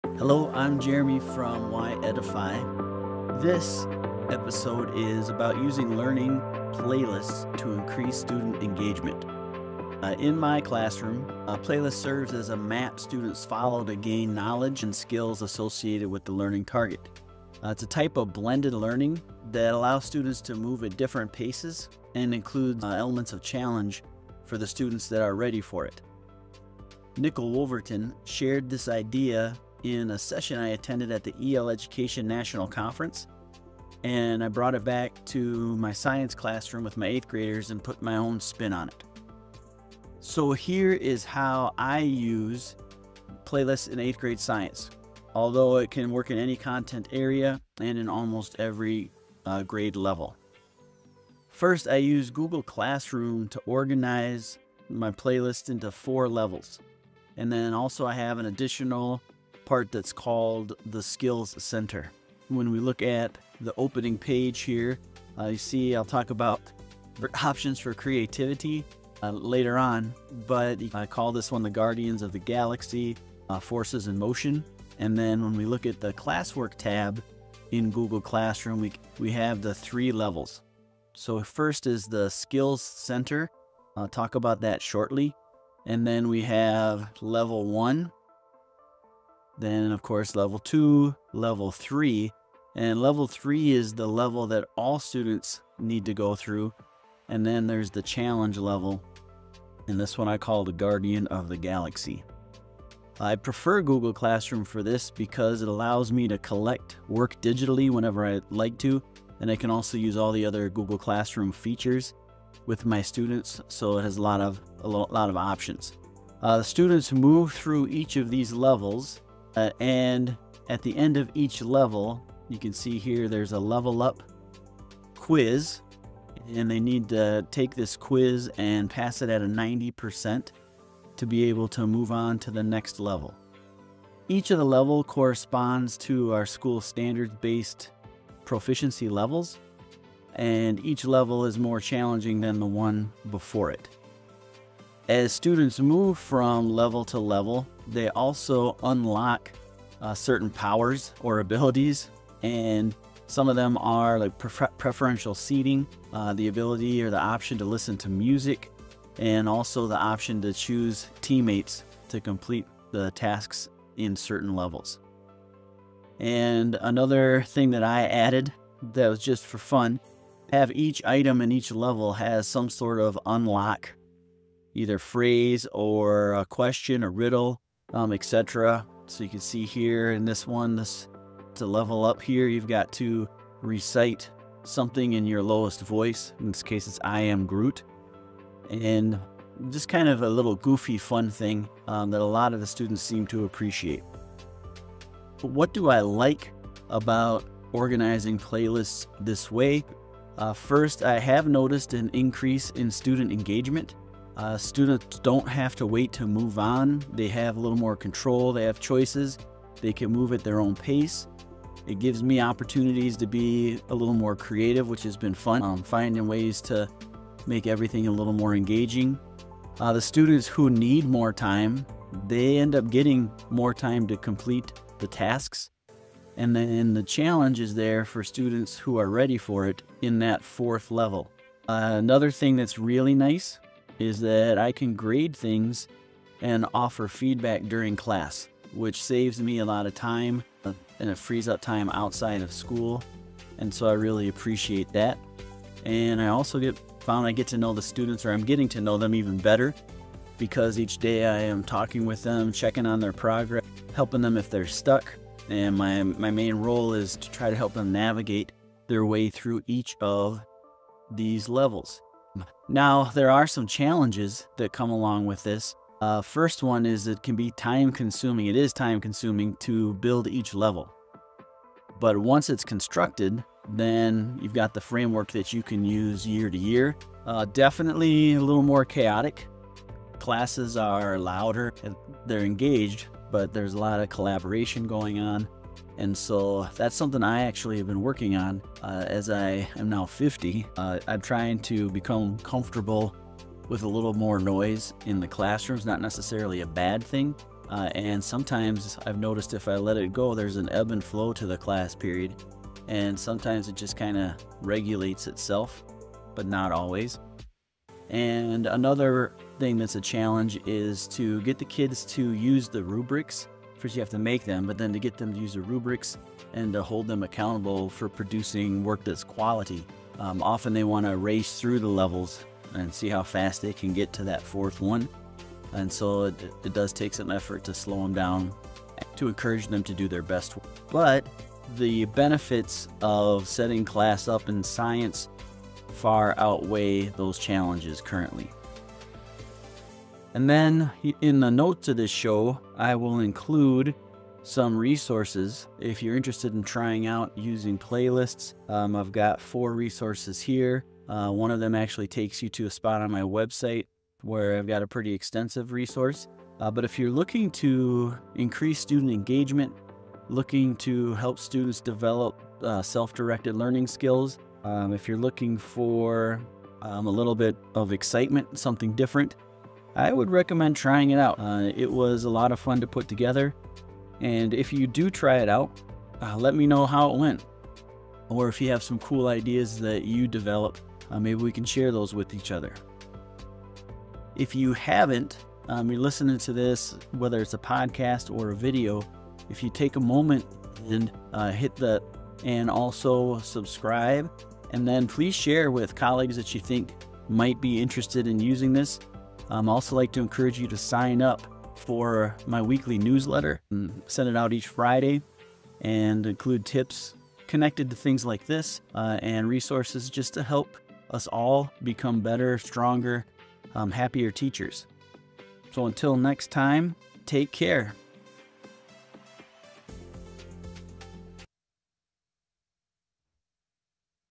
Boost Student Engagement and Personalize Learning with Classroom Playlists
Boost Student Engagement: Implementing Learning Playlists in the Classroom

In this episode, Jeremy from Y Edify discusses how to use learning playlists to enhance student engagement in the classroom. Jeremy explains his implementation of playlists in his eighth-grade science class, organizing them into four levels using Google Classroom. He shares the benefits and challenges of this blended learning approach, how it allows students to progress at their own pace, and the engaging elements they've incorporated.

Additionally, Jeremy offers practical tips and resources for teachers interested in adopting this method.

00:00 Introduction to Learning Playlists
00:40 Implementing Playlists in the Classroom
00:51 Organizing Playlists with Google Classroom
03:15 Student Engagement and Benefits
04:22 Challenges and Solutions
05:40 Resources and Final Thoughts